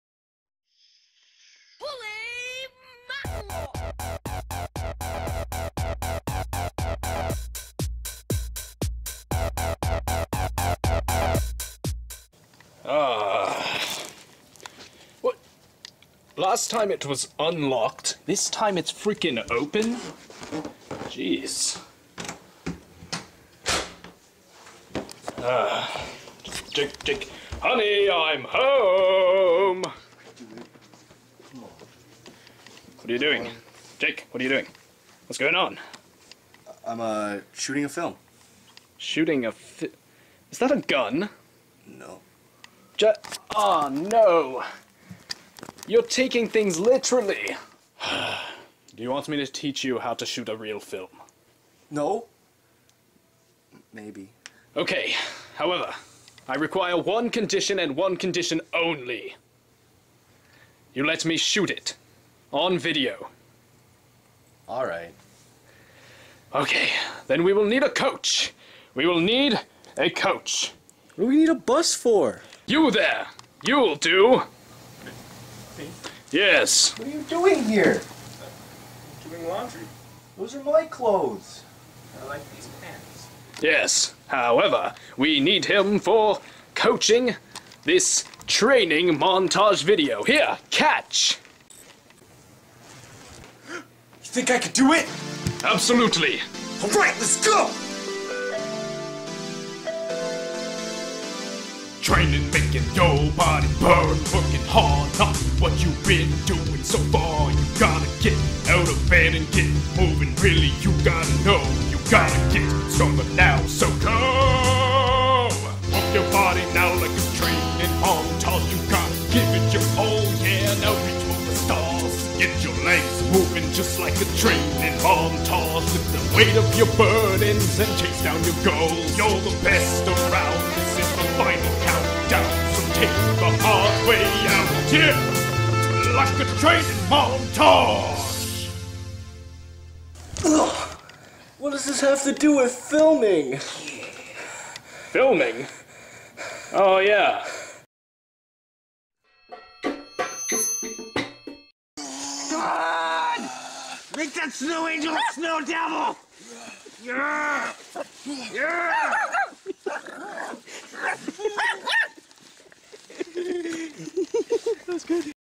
Exceedingly Stupid Episode 7 - Training Montage
This video sort of marks the beginning of our new(ish) music situation. From here on out, all of our music will be created from the ground up by us.